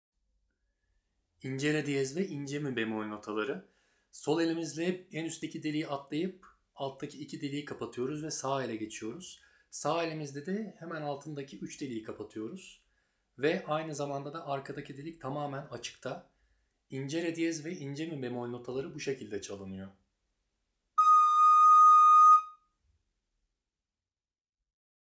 İnce Re Diyez(Re#*) - İnce Mi Bemol(Mib*) Blok Flüt ile Nasıl Çalınır?
Merhaba arkadaşlar, Ses Veriyorum'a hoş geldiniz.

Bu videoda İnce Re Diyez(Re#*) - İnce Mi Bemol(Mib*) notaları blok flüt ile nasıl çalınır anlatmaya çalıştım. Umarım faydalı olmuştur.

Herkese iyi çalışmalar...